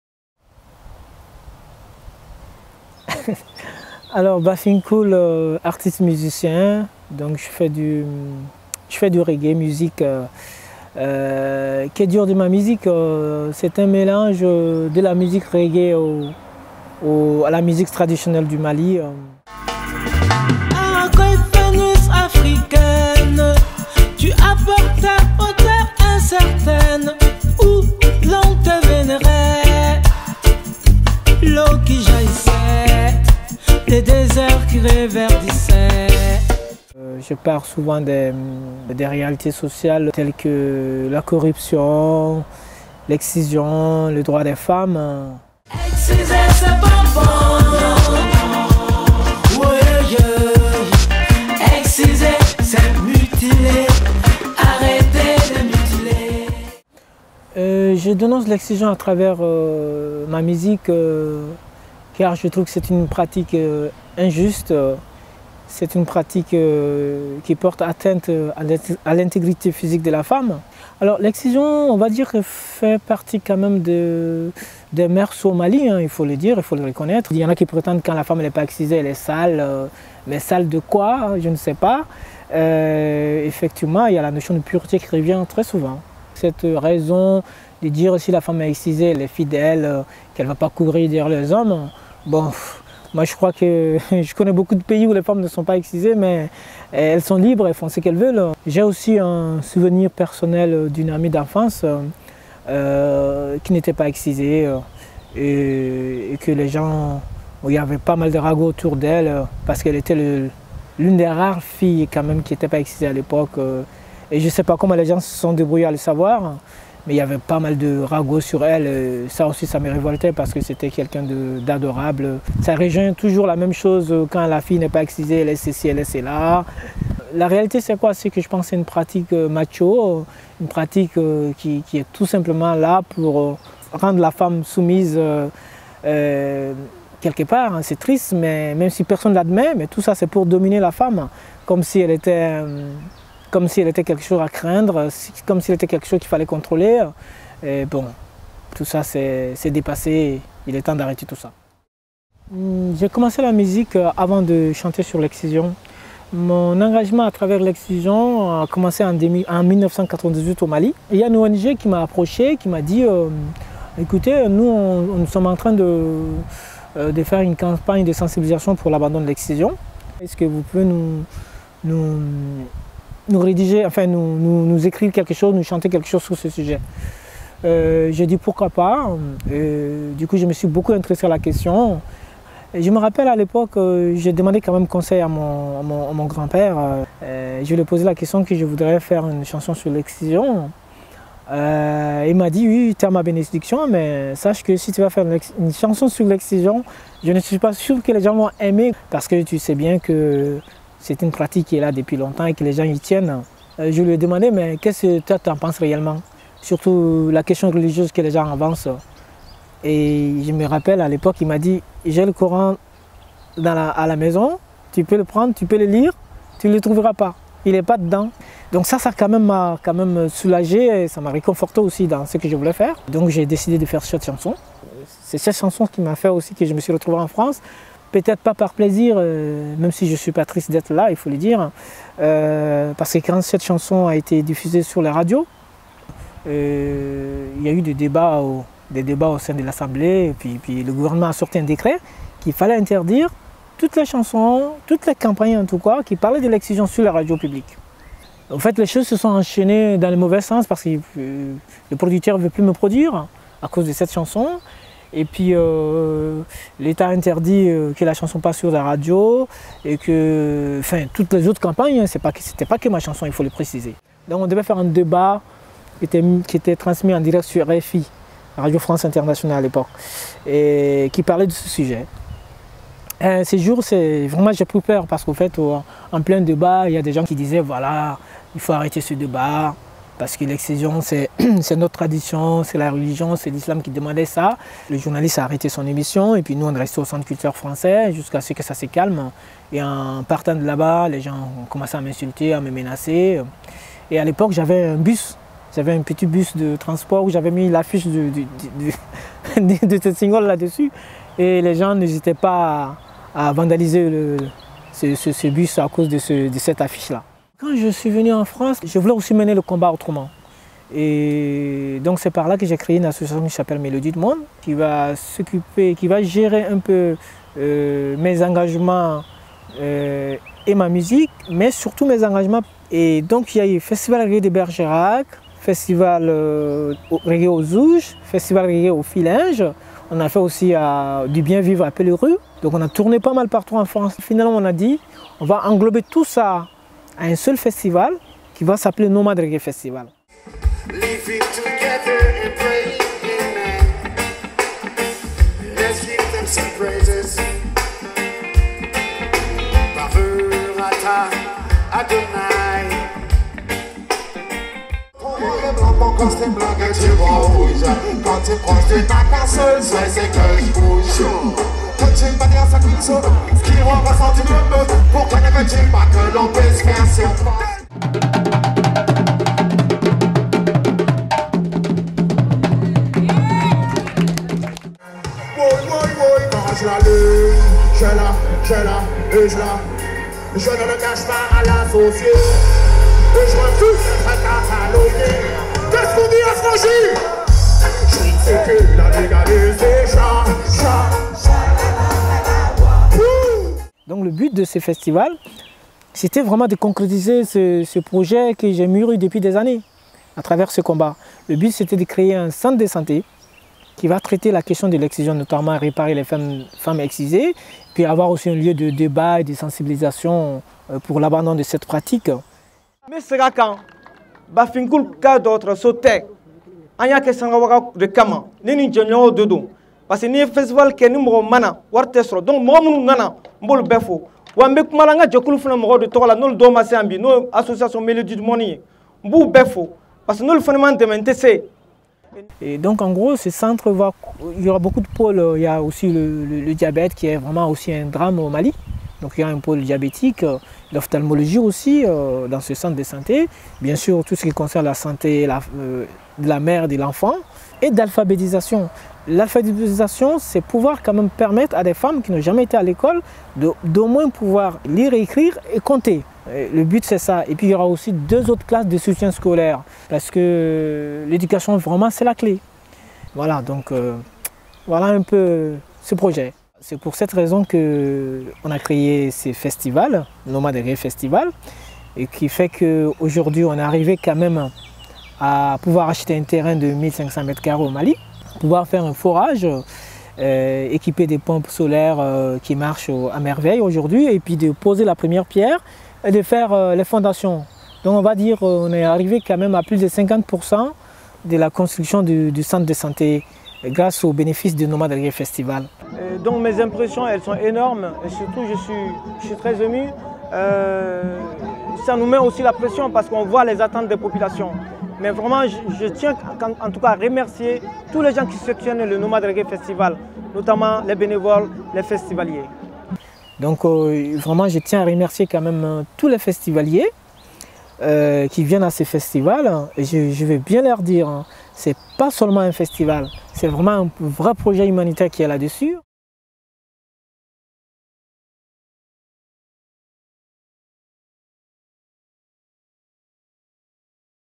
Bafing Kul, un musicien malien engagé contre l'excision
Compositeur et musicien, le chanteur malien Bafing Kul est engagé dans un combat qui lui tient particulièrement à cœur : la défense des droits des femmes et la lutte contre l’excision. Expatrié en France depuis 2002, il n’a pas pu continuer à chanter librement dans son pays natal. Et pour cause, il dénonçait l'excision, une pratique courante au Mali et peu contestée dans l'espace public. Bafing Kul poursuit désormais ses engagements en France. À la tête de l'association "Mélodies du Monde", cet artiste organise désormais des festivals en France dont les bénéfices serviront à la construction d'un centre socio-médical au Mali. Rencontre.

Interview réalisée par Rémy Lejeune & Anaïs Mariotti